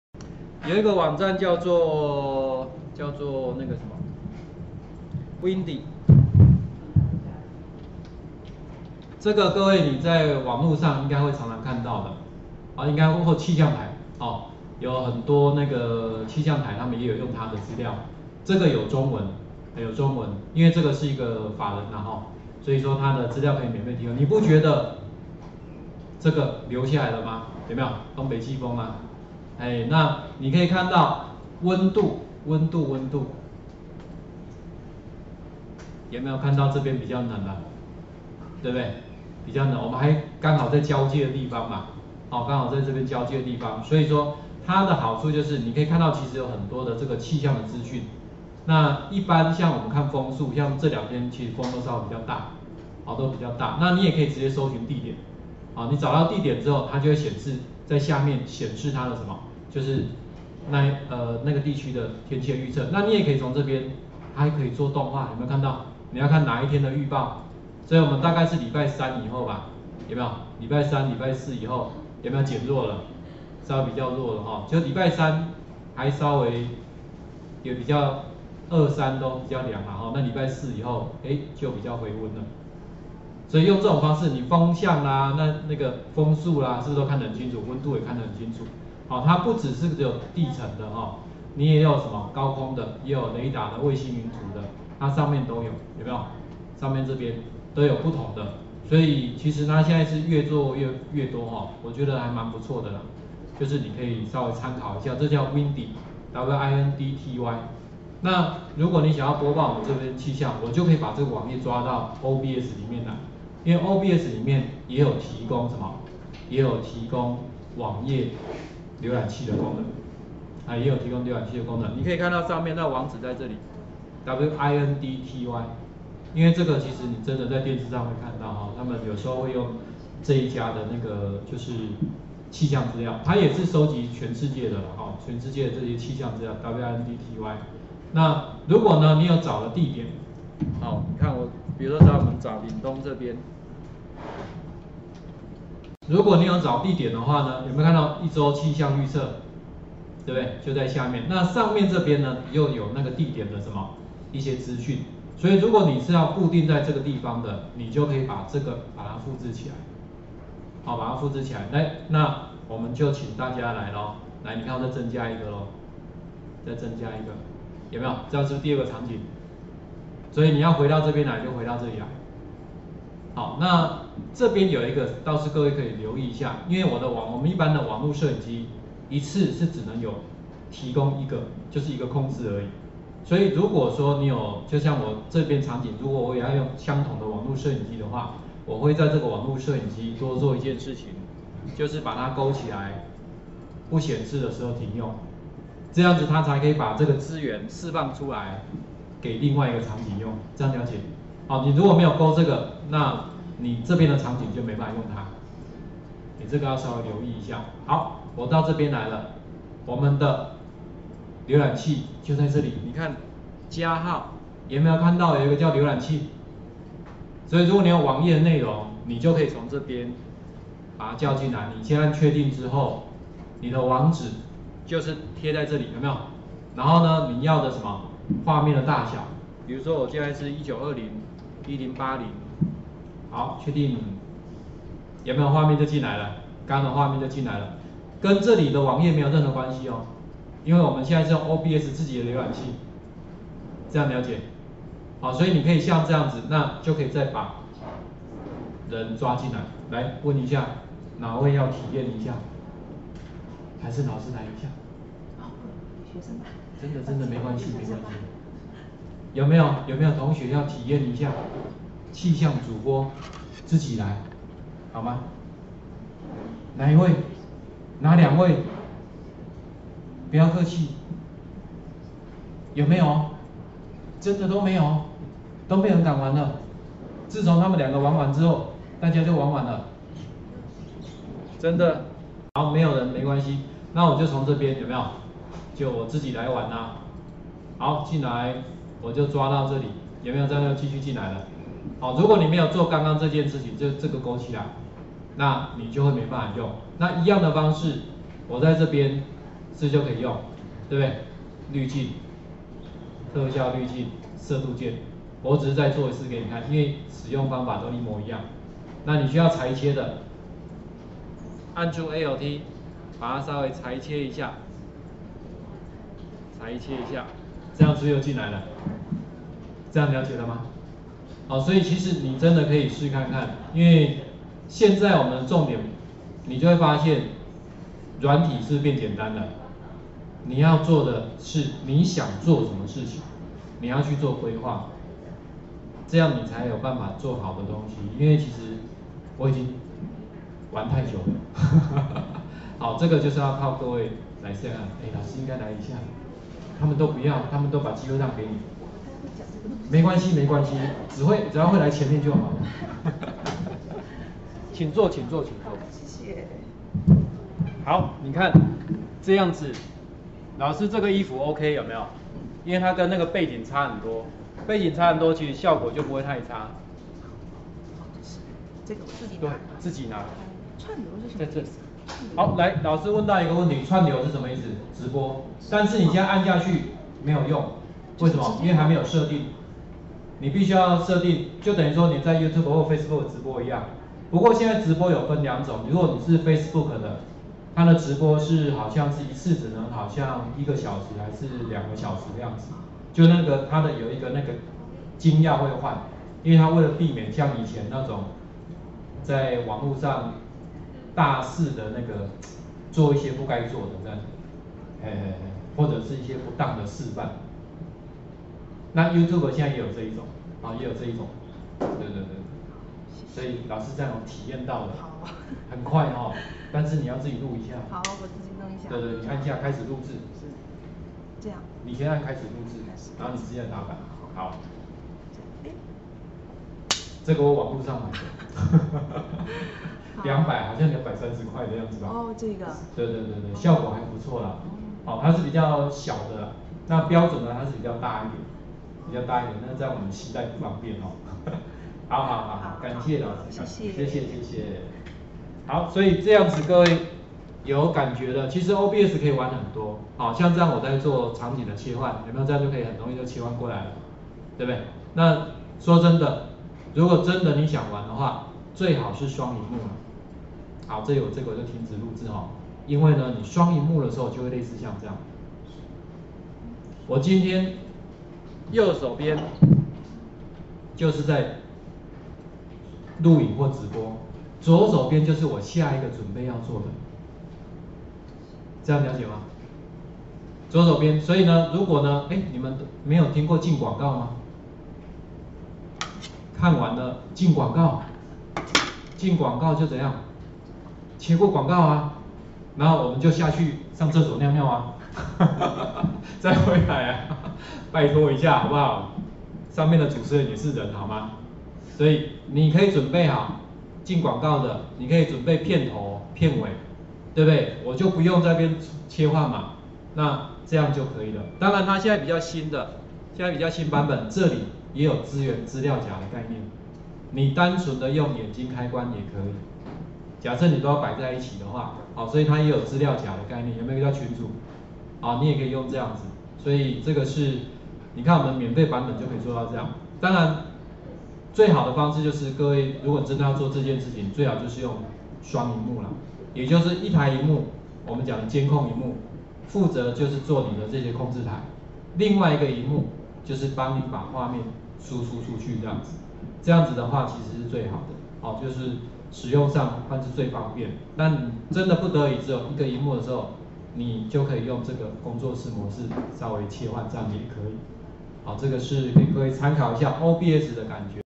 03：OBS 氣象主播體驗
此影片為AE範例「OBS 虛擬攝影棚與AE藍綠幕影片去背、RoroBrush影片去背筆刷介紹」歷史教學影片